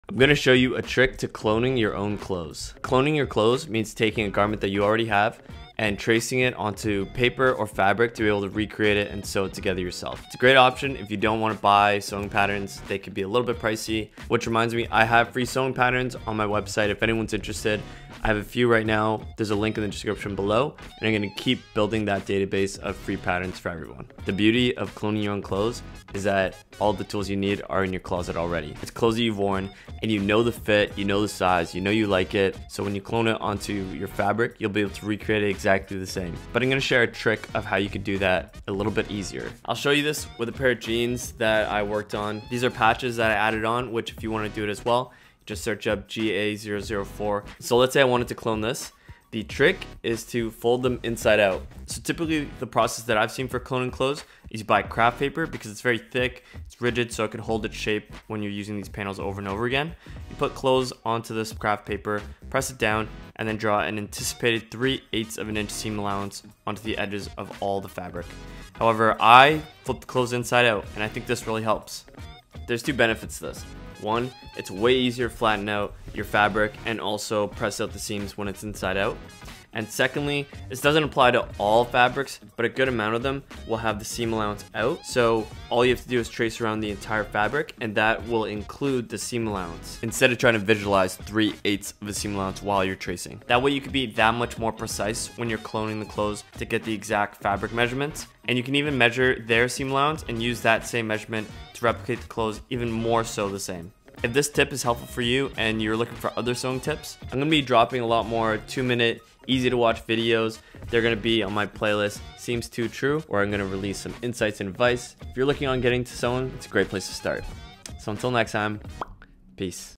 The Trick to Cloning Clothes | Seams Too True EP 1
Patternmaking trick for cloning your clothes.

🗣SOCIALS
Tag SharetheGlory in your photos